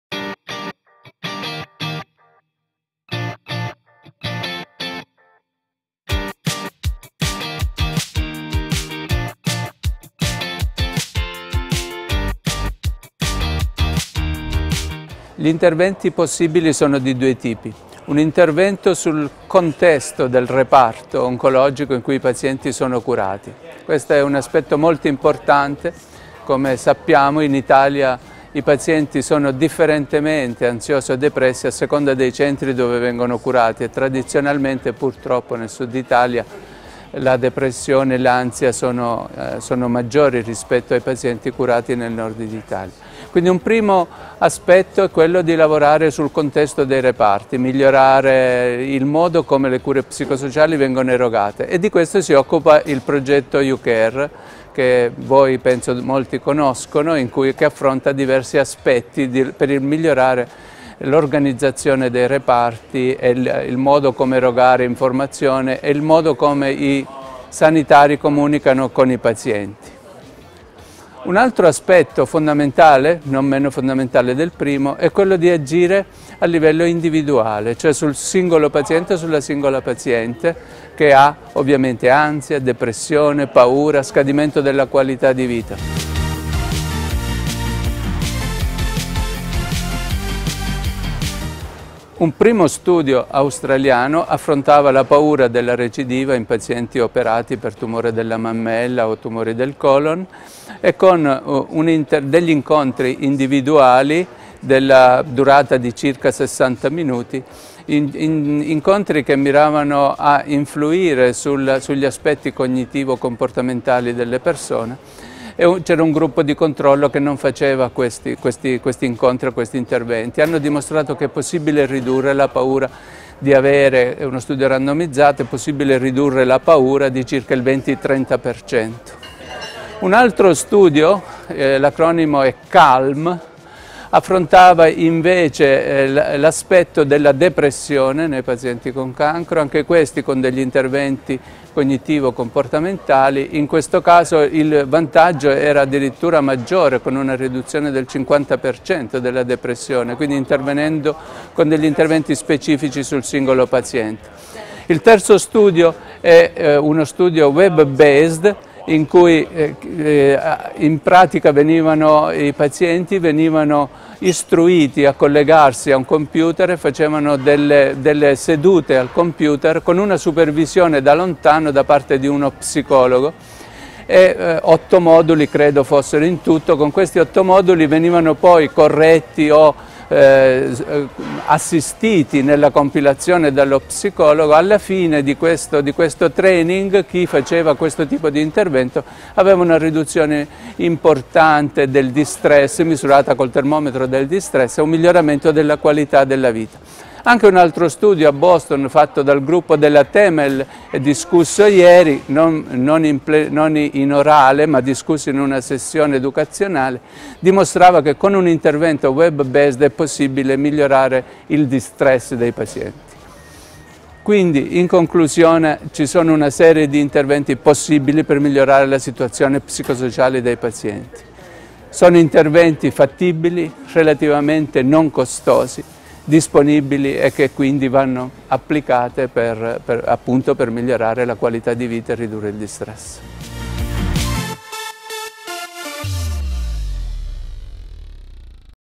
ASCO 2017: QoL e umanizzazione delle cure
Lo abbiamo chiesto a Rodolfo Passalacqua, Direttore della SC di Oncologia dell’Ospedale di Cremona in occasione dell’ASCO di Chicago.